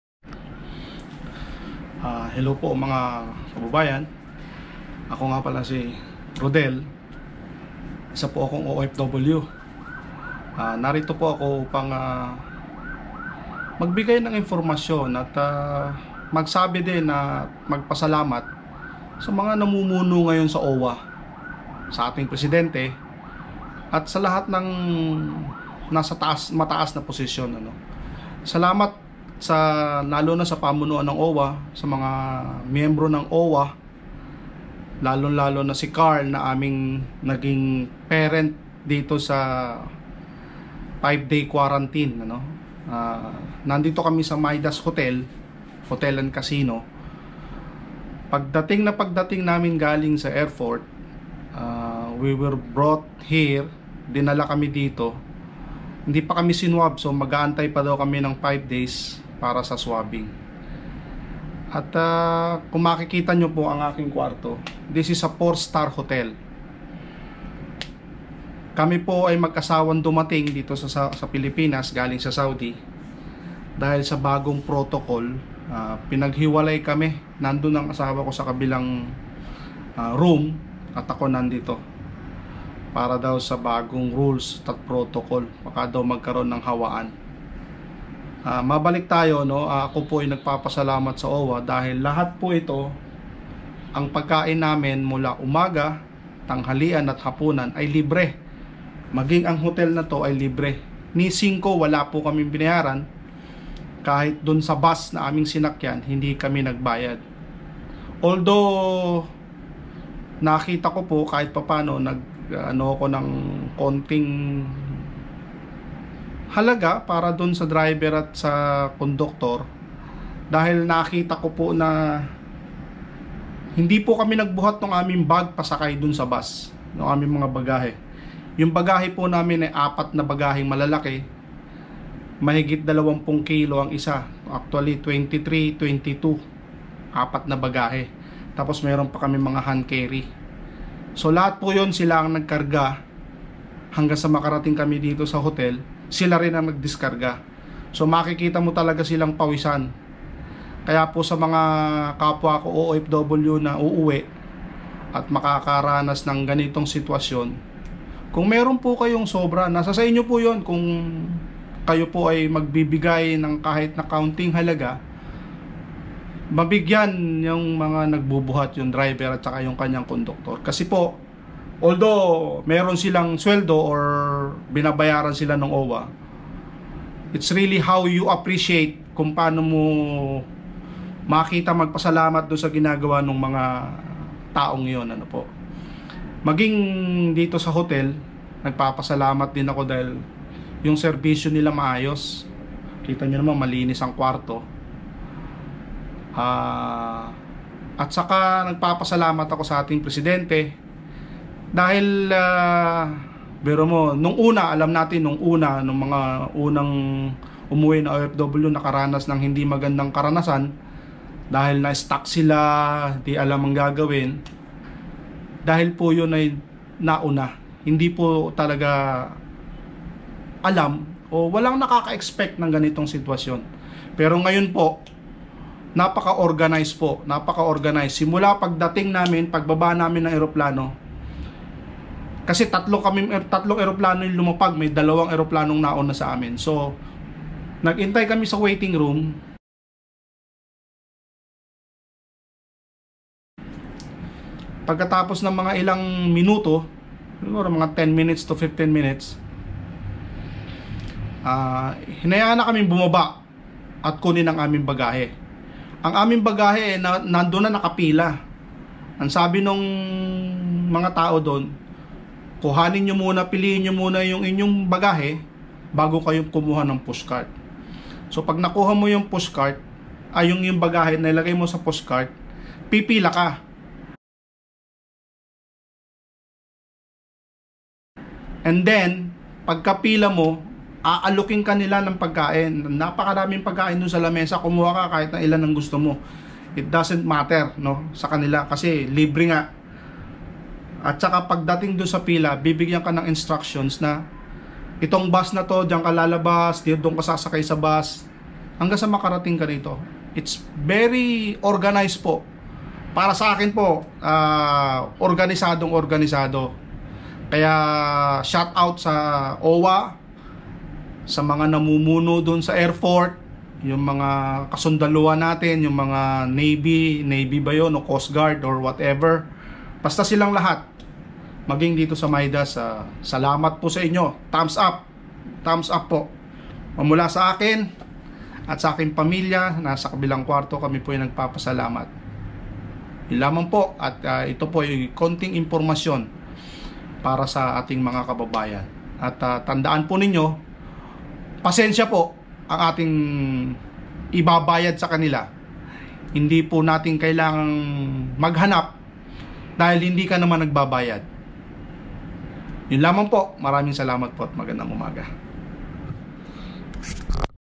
Thankful! OWWA CoastGuard MidasHotel
A Simple Information for all returning OFW.
Thankful sa OWWA at Philippine Coast Guard Personel
Specially sa ating Presidente Rodrigo Roa Duterte